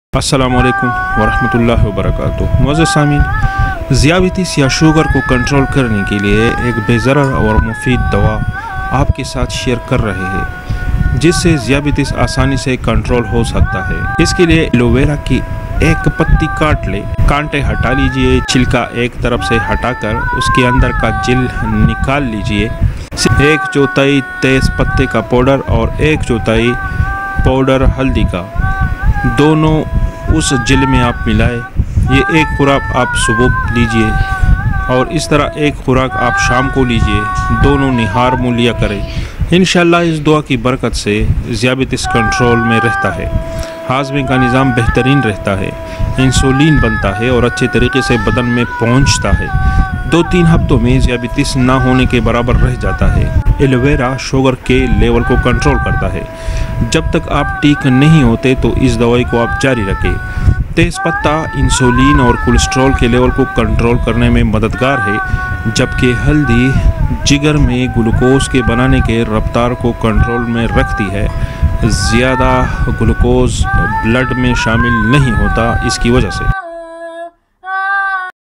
Special Home Remedy for Diabetes | How to Control Diabetes Naturally |sugar ka garilu ilaj
Ais video mai sugar ka zabardast herbal ilaj hai Inshallah Aap ko faida hoga.